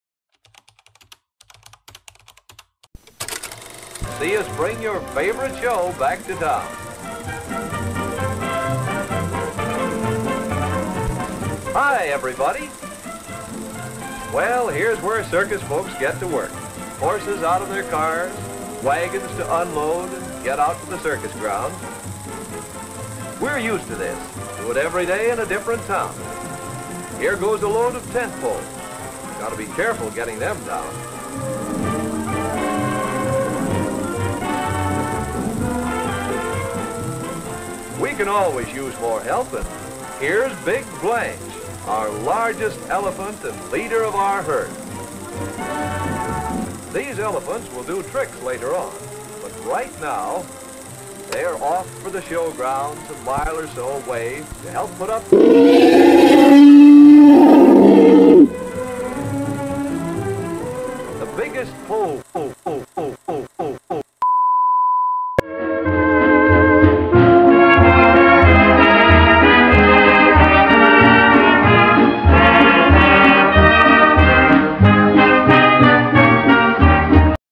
Elephants{Analogue horror trailer}
Hello Everyone! This is a sort of teaser short video for this Analogue horror series I'm working on. It's my first time making a project like this and I would love if people would share their ideas and opinions in the comments :)